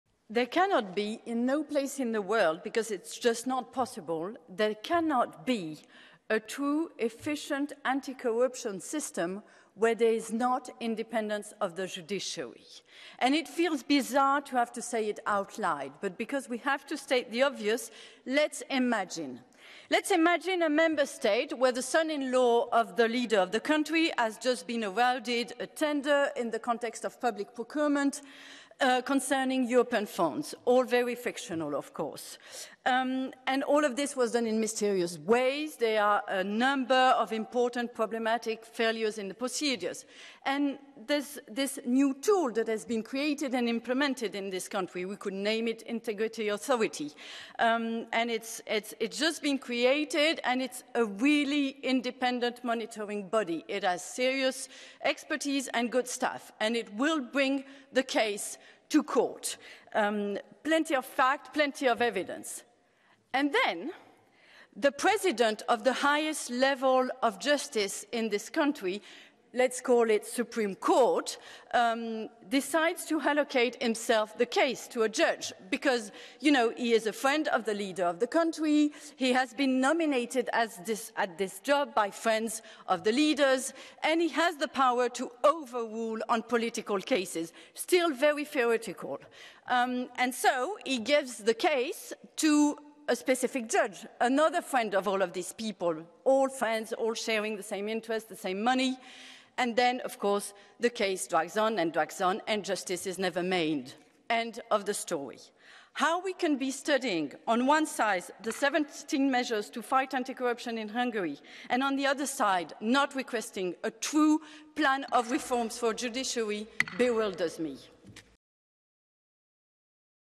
Gwendoline Delbos-Corfield sur l'évaluation du respect de l'État de droit par la Hongrie
Lundi 21 novembre, Gwendoline Delbos-Corfield est intervenue sur l'évaluation du respect par la Hongrie des conditions relatives à l'état de droit prévues par le règlement relatif à la conditionnalité et état d'avancement du Plan de relance et de résilience (PRR) hongrois.
Son intervention ⬇️

"Nulle part dans le monde, il ne peut y avoir, parce que c'est impossible... Il ne peut y avoir un système anticorruption efficace quand le système judiciaire n'est pas indépendant. Et c'est étrange d'avoir à le dire, mais puisqu'il faut rappeler les évidences, imaginons.
Imaginons un État membre où le gendre du chef d'État s'est vu attribuer un marché dans le cadre de la commande publique en lien avec des fonds européens. Bien sûr, tout ça est fictif. Tout ça s'est fait de manière très mystérieuse. Il y a un certain nombre de défaillances problématiques dans la procédure.
Et il y a ce nouvel instrument, qui a été mis en place dans ce pays. Appelons-le "organisme déontologique". Il vient tout juste d'être créé. C'est un organisme de surveillance réellement indépendant qui a une grande expertise et d'excellents employés. Et qui portera cette affaire devant les tribunaux. Beaucoup de faits. Beaucoup de preuves.
Et là, le président de la plus haute juridiction du pays, appelons-la Cour suprême, décide de choisir lui-même le juge parce que c'est un ami du chef d'État, qu'il a été nommé à ce poste par des amis du chef d'État et il a le pouvoir de décision dans les affaires politiques.
Toujours en théorie, il donne le dossier à un juge précis. Un autre ami de tous ces gens. Tous amis, tous avec les mêmes intérêts, avec le même argent. Et, bien sûr, le dossier traîne en longueur et la justice n'est jamais rendue.
Fin de l'histoire.
Comment peut-on, d'un côté, étudier les 17 mesures pour lutter contre la corruption en Hongrie et, d'un autre côté, ne pas demander un vrai plan de réforme de la justice ? Ça me dépasse."